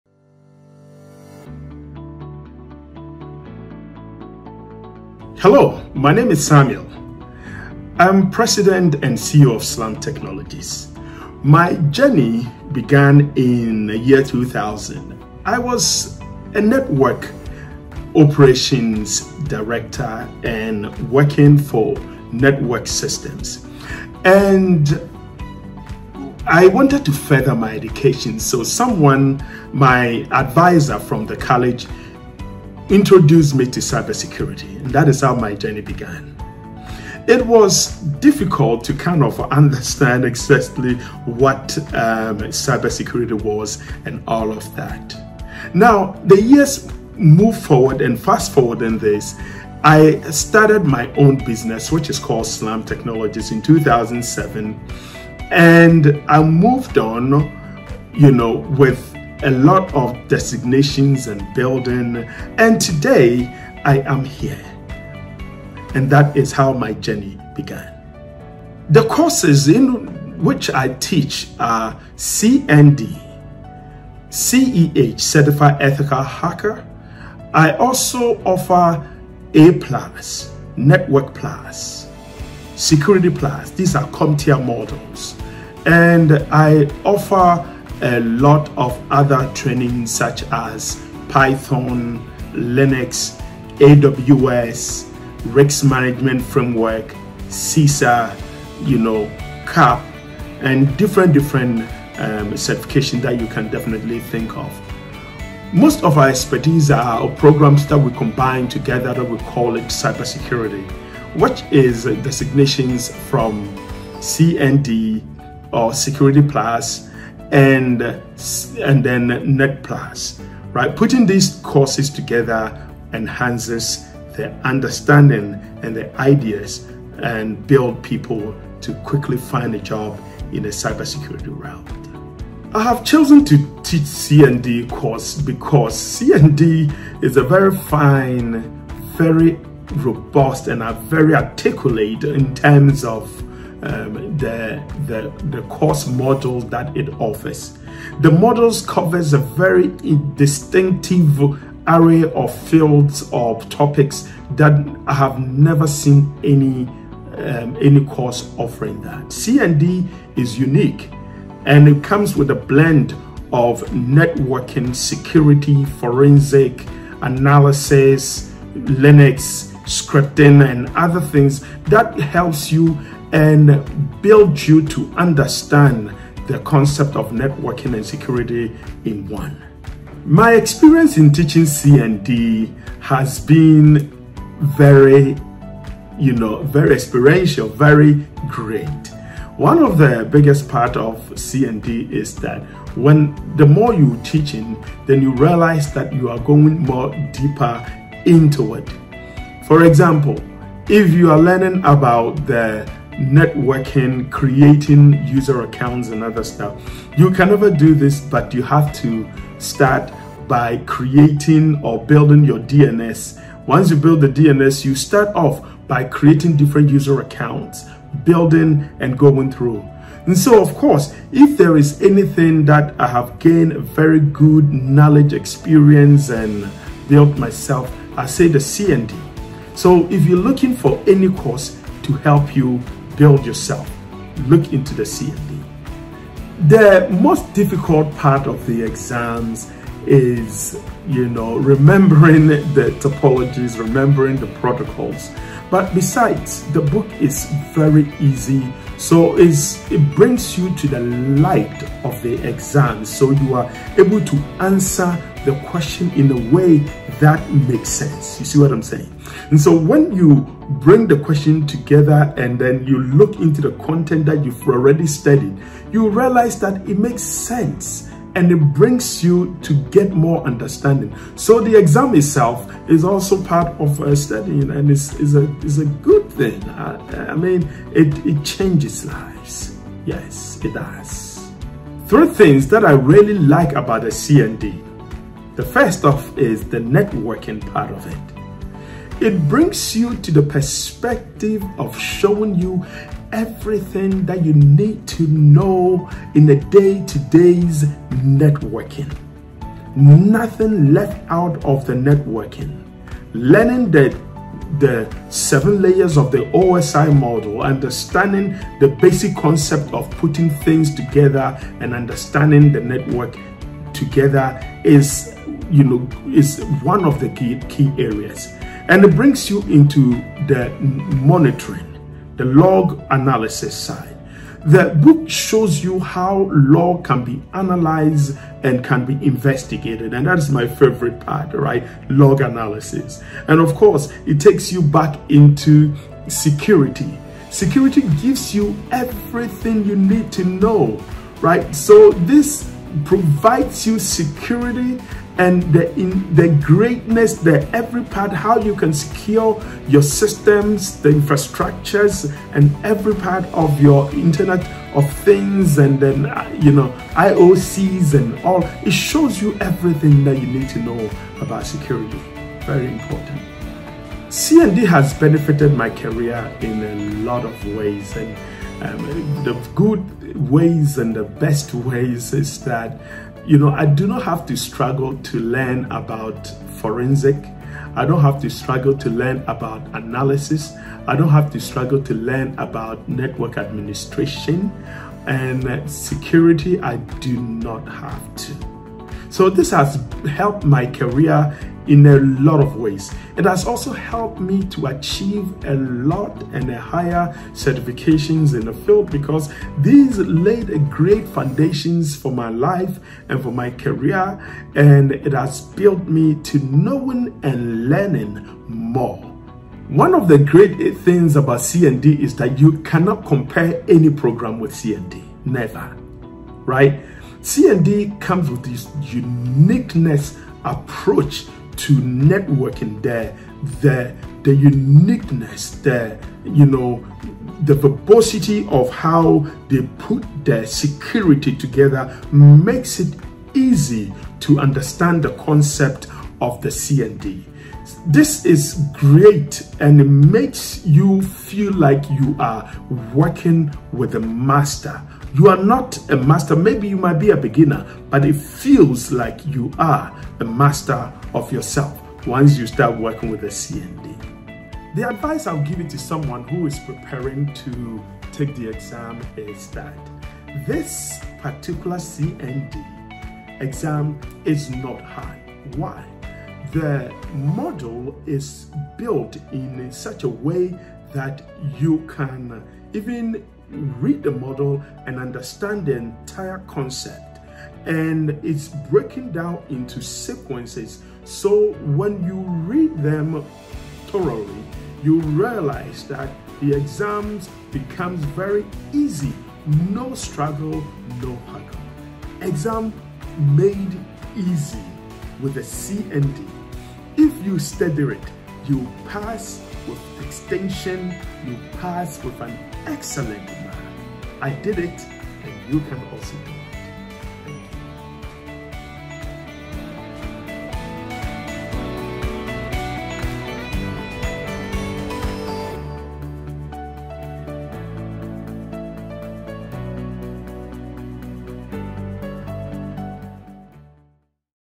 Journey As A Cybersecurity Professional | Samuel Boateng's Success Story With CND
In this video Samuel Boateng, the President and CEO of Slamm Technologies shares his experience on getting certified in C|ND and becoming an instructor.

His journey in cybersecurity began in 2000 as per the recommendation of his college advisor. At first, the domain appeared highly challenging to him. However, in a few years, he went on to set up a global IT firm. The organization commenced its operations in 2007.

As an EC-Council instructor, Samuel teaches C|ND and C|EH. He chose to teach the Certified Network Defender (C|ND) program because he finds the course to be robust with refined modules. According to Samuel, C|ND is unique due to its in-depth coverage of topics and comprehensive course materials, and is unparalleled to other certifications. He describes his C|ND teaching experience as “fulfilling” and believes that it contributes significantly by helping professionals gain skills and build careers in cybersecurity.

C|ND is a blend of networking, security, forensics, analysis, and Linux, and equips learners with a comprehensive understanding of network security and defense. While the most challenging aspect of C|ND exam is memorizing the protocols, the learning materials provided by C|ND provide better clarity of concepts to the learners.

Timestamp
0:03: Samuel’s cybersecurity career
1:09: Cybersecurity courses taught by Samuel Boateng
2:13: Reasons for teaching the C|ND course
3:01: Experience with teaching the C|ND certification
3:57: Challenging parts of the C|ND exam
4:44: Three interesting aspects of the C|ND certification
6:06: How C|ND benefitted Samuel’s career
6:50: C|ND compared to other programs
7:35: Advice to those interested in taking C|ND exam